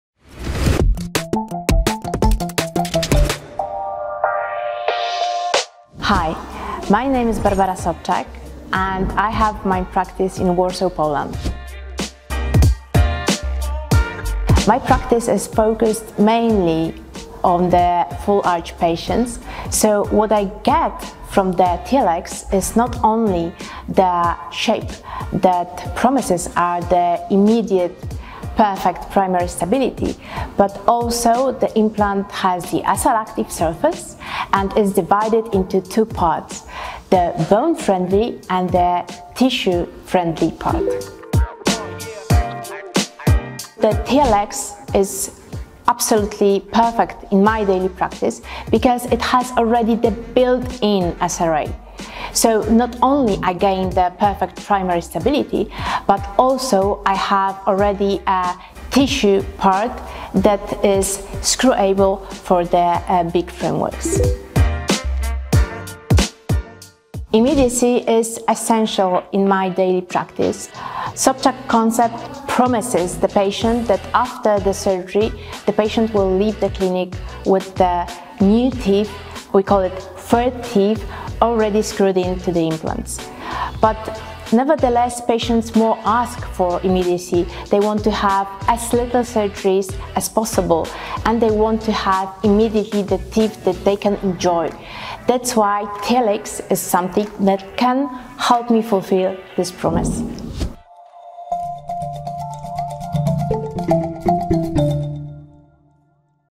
60 SECONDS with Dr. Barbara Sobczak on the Straumann® Pro Arch TLX
The new Straumann® TLX Implant System is the next stage of evolution for Tissue Level implants. In this episode of 60 SECONDS, Dr. Barbara Sobczak shares her experience about the importance of immediacy in her practice and how the new implant system supports in it.